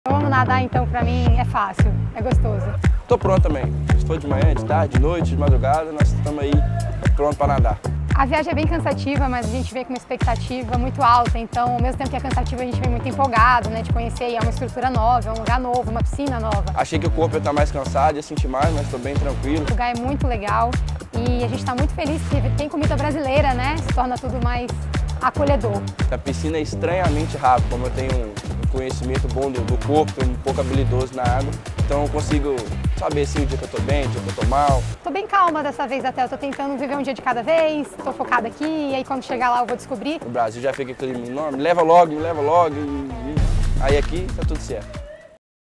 Aclimatação Troyes - Primeiros treinos da natação
Já tem muita natação em Troyes para a nossa delegação brasileira! Os atletas conheceram a piscina, começaram a sentir o clima dos Jogos Paralímpicos e aproveitaram cada detalhe. A Mariana Gesteira e o Gabriel Araújo estão aqui para provar isso!